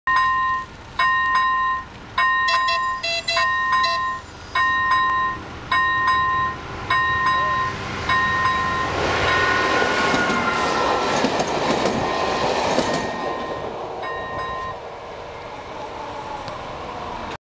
Catr 854: 854 027 0 Lenka, projíždí železničním přejezdem ve Kbelích
Lenka projíždí železničním přejezdem ve Kbelích.
okomentujte, ohodnoťte, sdílejte, popřípadě odebírejte
velmi děkuji za shlédnutí